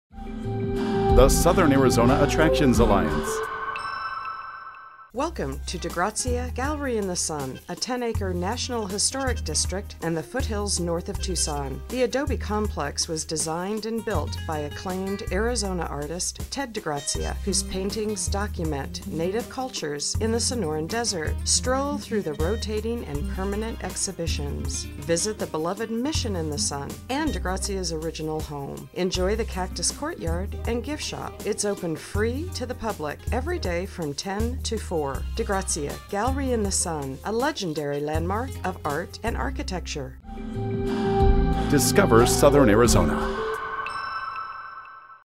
DeGrazia Gallery in the Sun
National Historic District of legendary art and architecture created by world-renowned Arizona artist Ted DeGrazia. The 10-acre foothills retreat includes the sprawling adobe gallery, gift shop, cactus courtyard, Mission in the Sun, artist's former home, and his grave. Six permanent collections of DeGrazia originals trace the history of colorful cultures of the Sonoran desert, and rotating exhibits include a wide range of mediums that include oil paintings, watercolors, ceramics and sculptures. Open daily from 10 a.m.-4 p.m. with free admission.